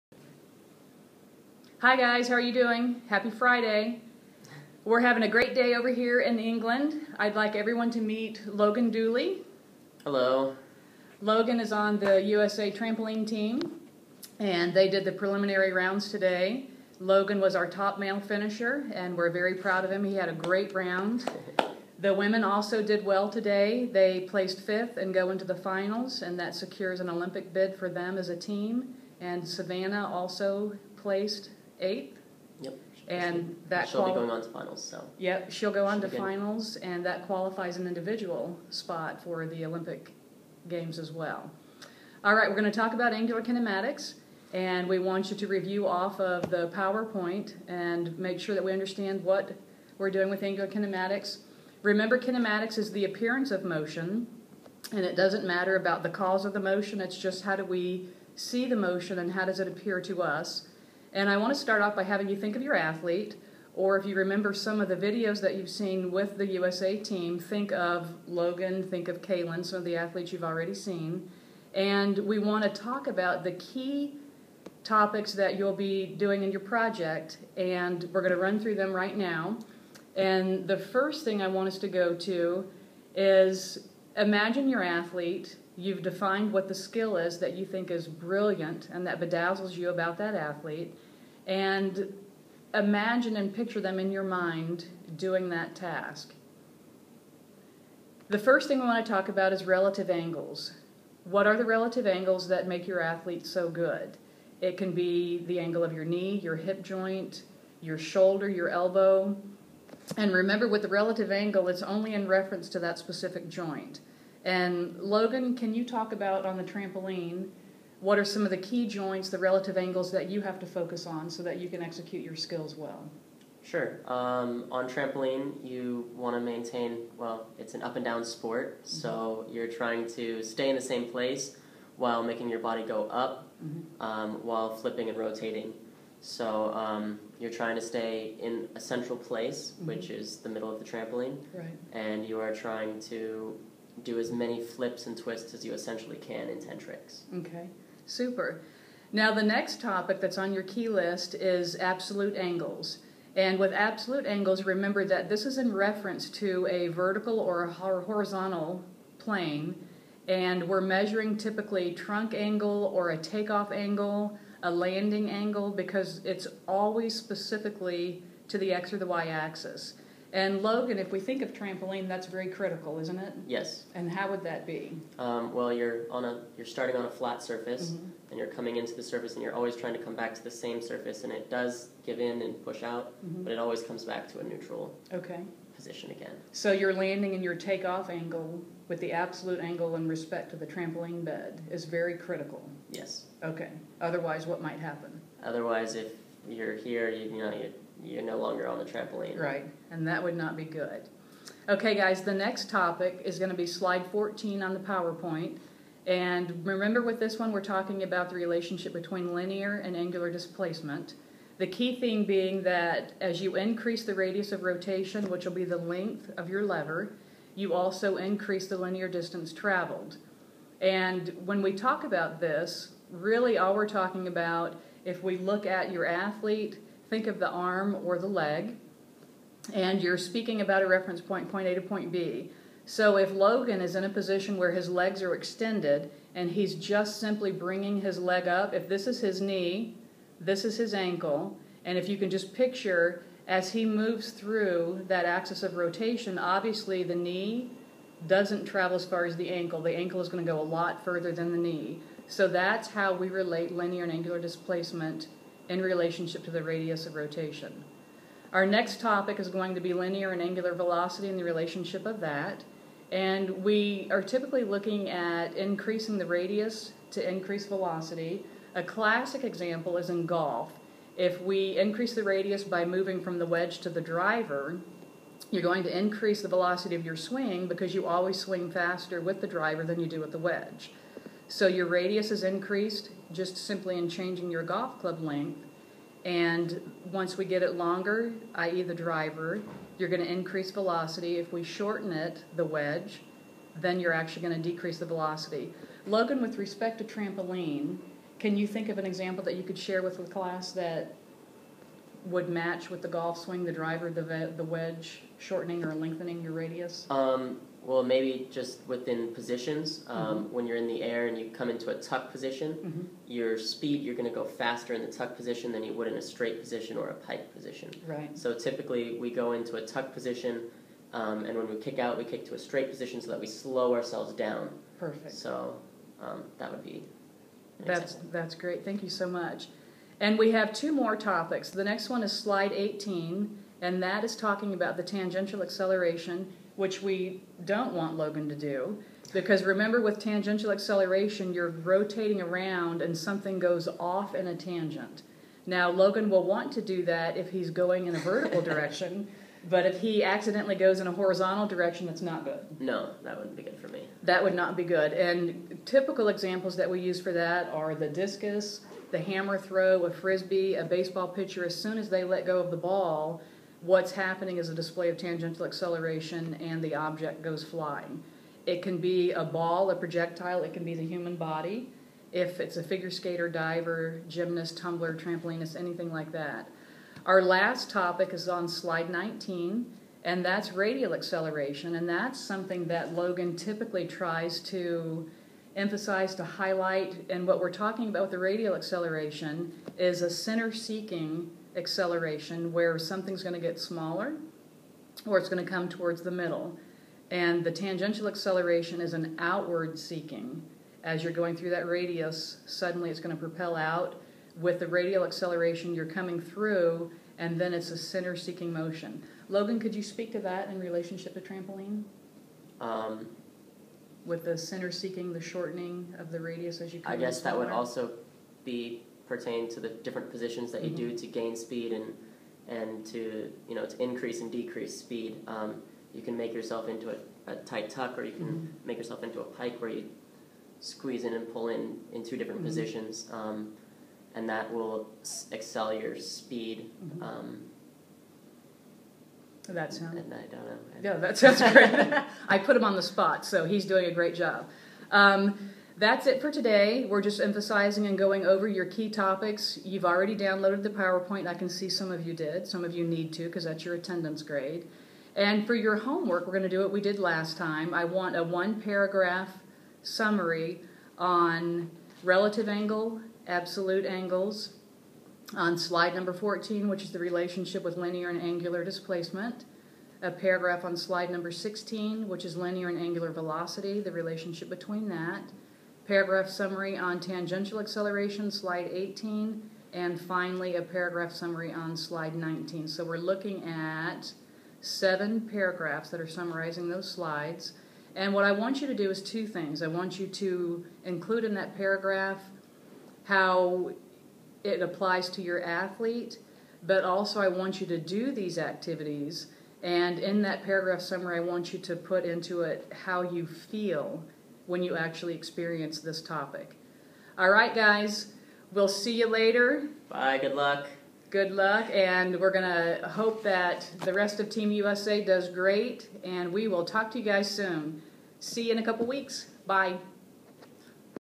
FSC Angular Kinematics
FSC's Dr Nancy H Cummings teaching Angular Kinematics at the World Championships